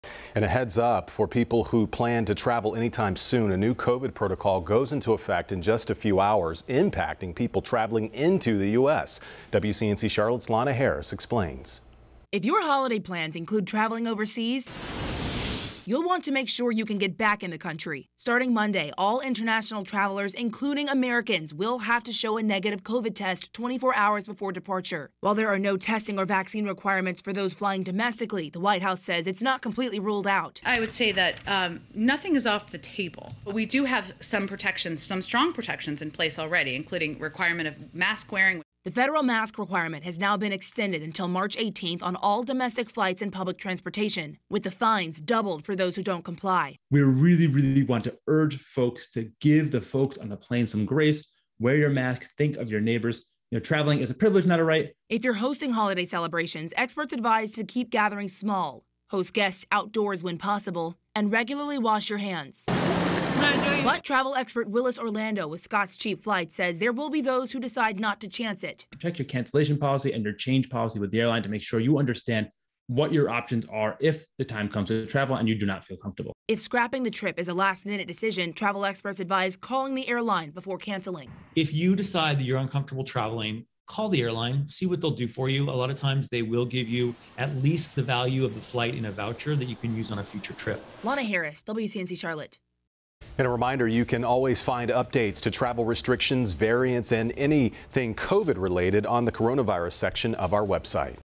New COVID-19 rules for international air travel start Dec. 6. Here's what you need to know.
If your holiday plans include traveling overseas, you'll want to make sure you can get back in the country.

Starting Monday, Dec. 6, all international travelers, including Americans, will have to show a negative COVID-19 test 24 hours before departure. While there are no testing or vaccine requirements for those flying domestically, the White House says it's not completely ruled out.

"Nothing is off the table," White House Press Secretary Jen Psaki said. "But we do have some strong protections in place, already including the requirement of mask wearing."

The federal mask requirement has been extended until March 18 for all domestic flights and public transportation, with the fines doubled for people who don't comply.